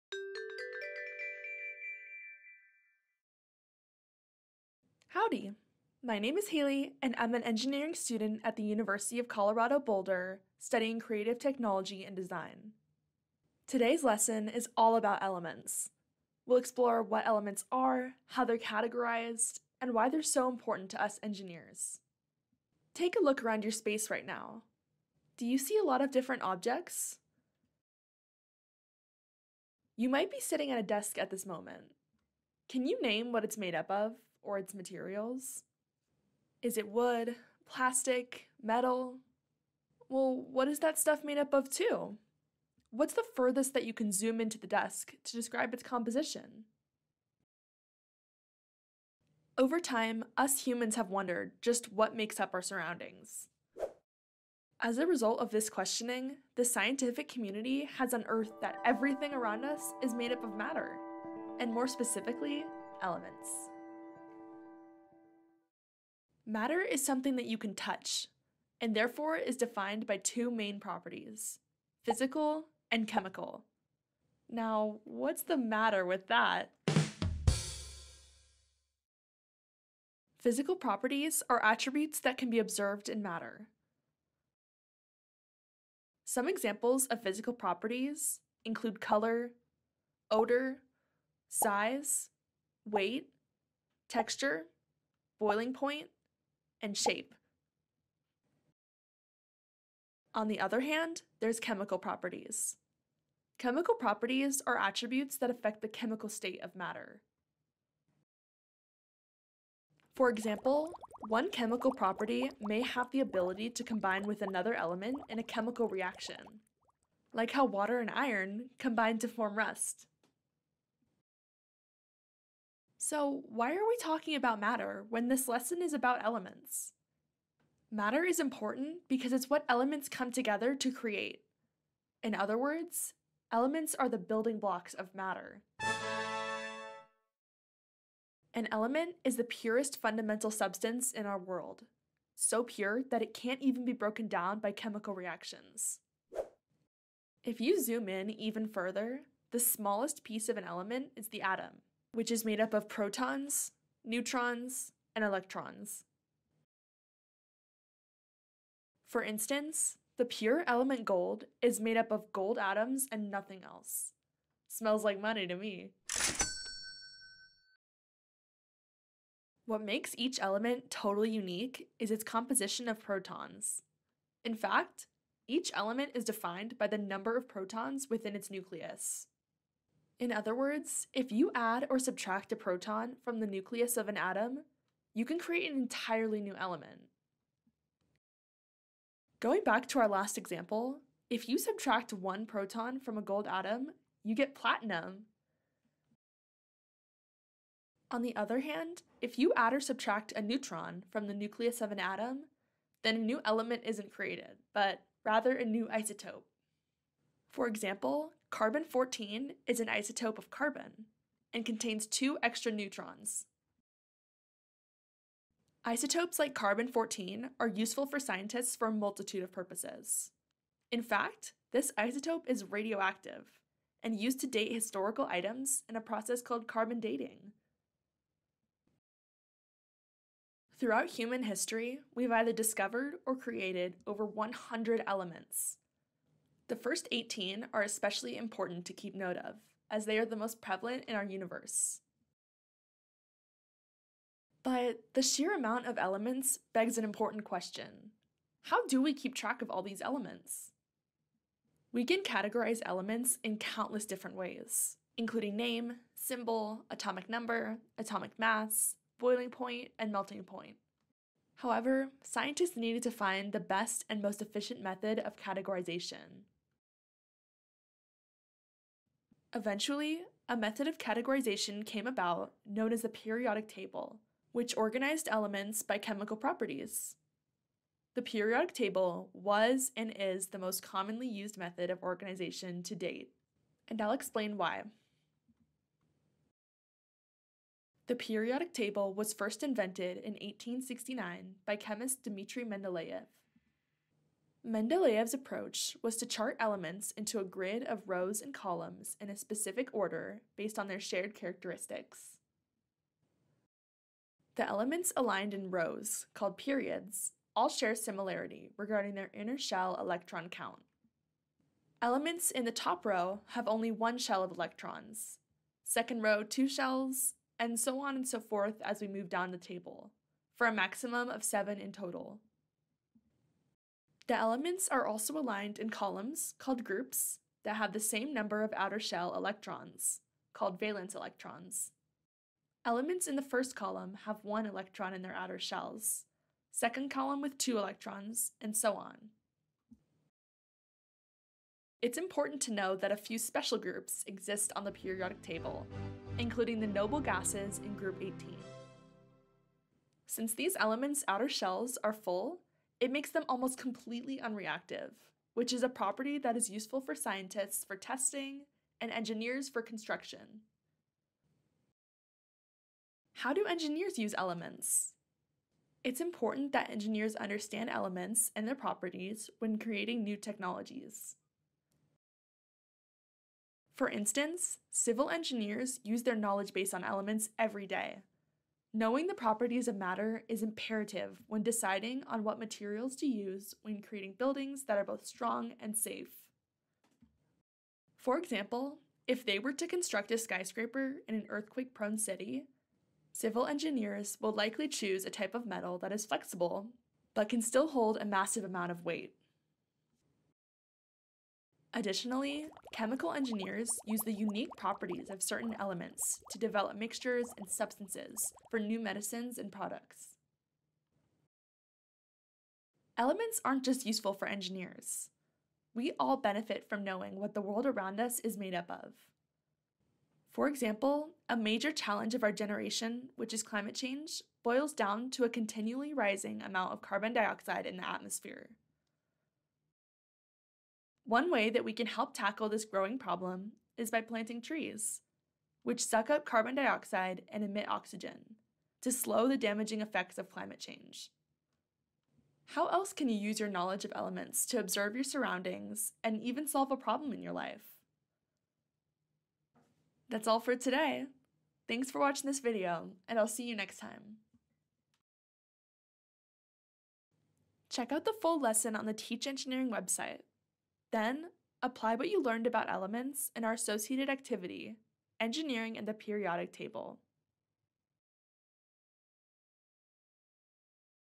Understanding Elements
Students examine the periodic table and the properties of elements. They learn the basic definition of an element and the 18 elements that compose most of the matter in the universe. The periodic table is described as one method of organization for the elements. The concepts of physical and chemical properties are also reviewed.

TeachEngineering has over 1,500 FREE lessons and activities. for more